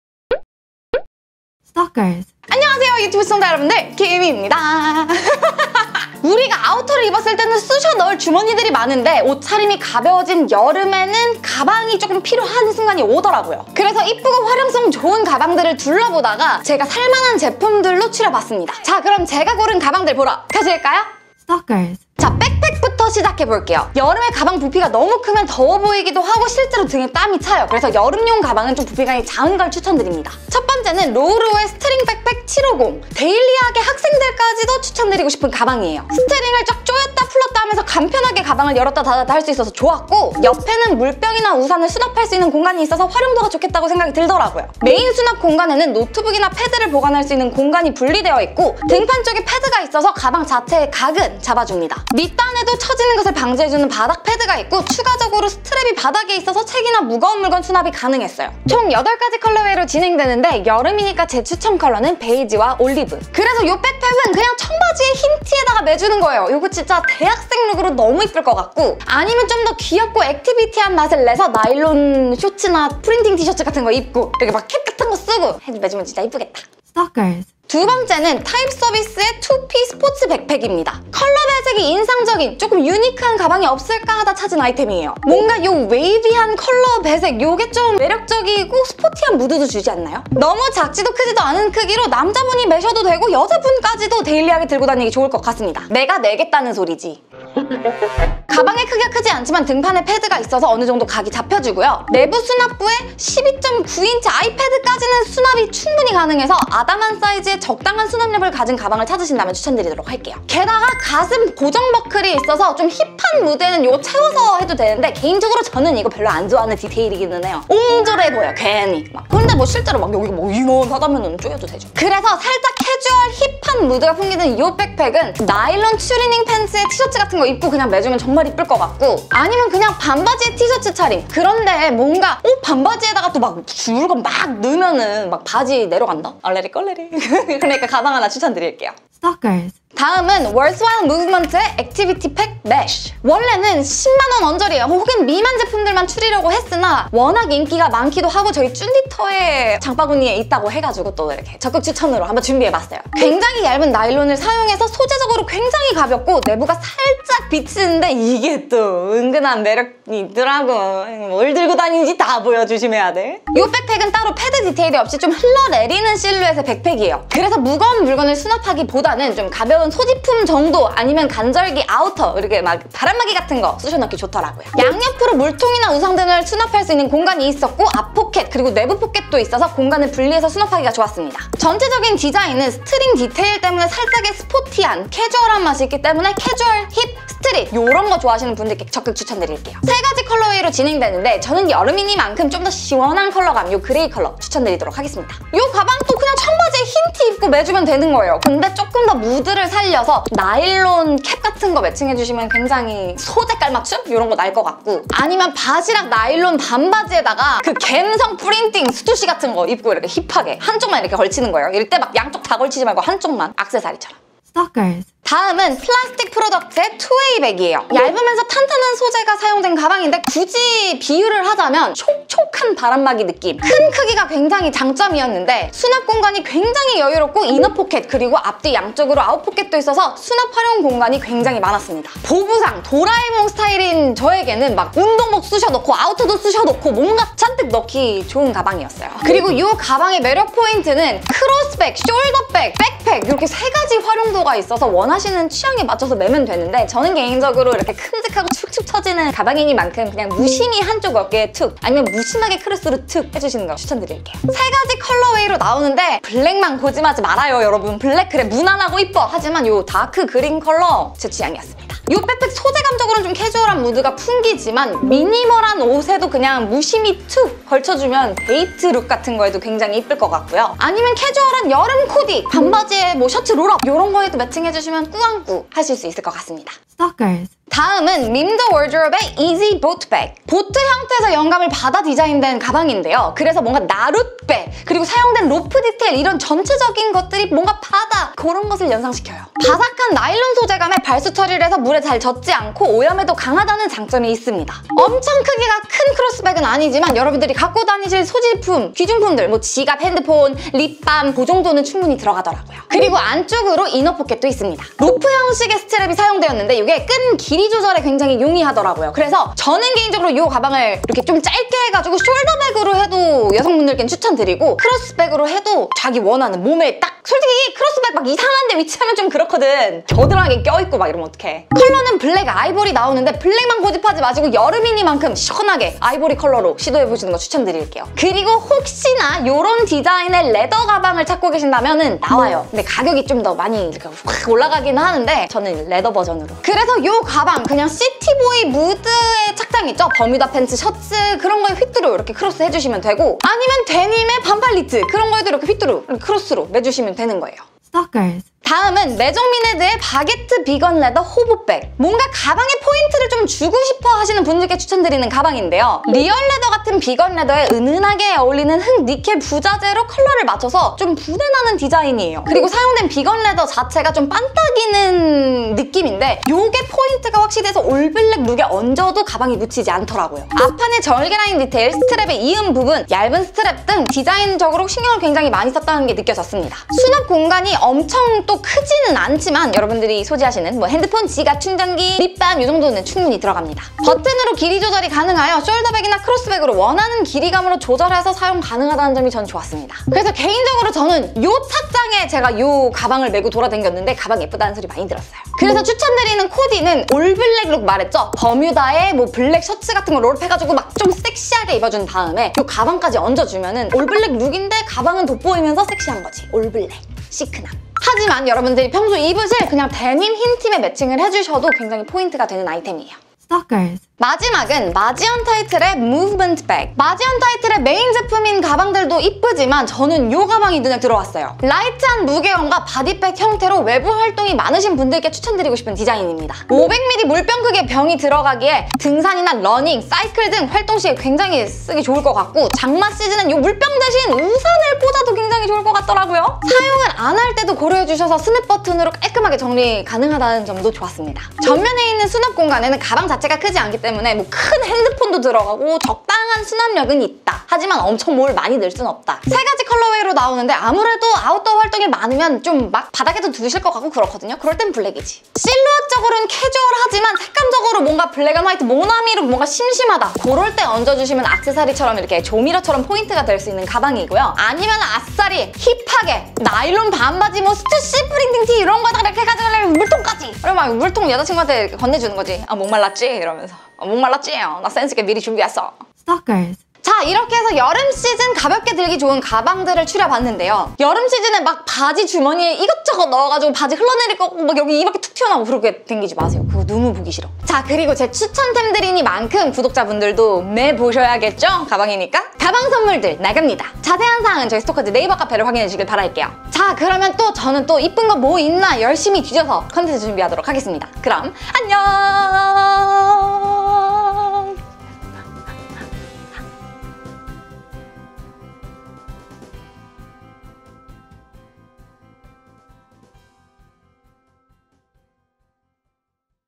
[대량리뷰] 갓성비 남자 가방 종류별 추천 + 코디
**오늘 영상은 제가 한 제품, 한 제품 셀렉해서, 브랜드 요청을 드려서,
   금전지원 없이 제품 지원만 받아서 만들어졌습니다**
*영상 속 코디 아이템의 정보는 아래 링크를 참고해주세요!

오늘은 여름에 포인트로 하기 좋은
가볍고 편한 가방을 준비했습니다!

백팩과 크로스백 2가지 모두 준비했으니
취향에 맞는 아이템으로 여름 코디에 활용해보셨으면 좋겠습니다ㅎㅎ

1. [로우로우] STRING BACKPACK 750 BEIGE

2. [타입서비스] 2P Sports Backpack [Gray]

3. [월스와일무브먼트] ACTIVITY PACK Mesh (Pure grey crease)

4. [플라스틱 프로덕트] TWO WAY BAG (DEEP GREEN)

5. [밈더워드로브] EASY BOAT BAG_BLACK

6. [메종미네드] BAGUETTE VEGAN LEATHER HOBO BAG

7. [마지언타이틀] movement bag (black)

0:00 오프닝
0:21 로우로우 RAWROW
1:16 타입서비스웹 TypeServiceWeb
2:19 월스와일 무브먼트 Worthwhile Movement
3:34 플라스틱 프로덕트 PLASTIC PRODUCT
4:37 밈더워드로브 MIM the wardrobe
5:47 메종미네드 MAISON MINED
6:53 마지언타이틀 mazi untitled
7:56 엔딩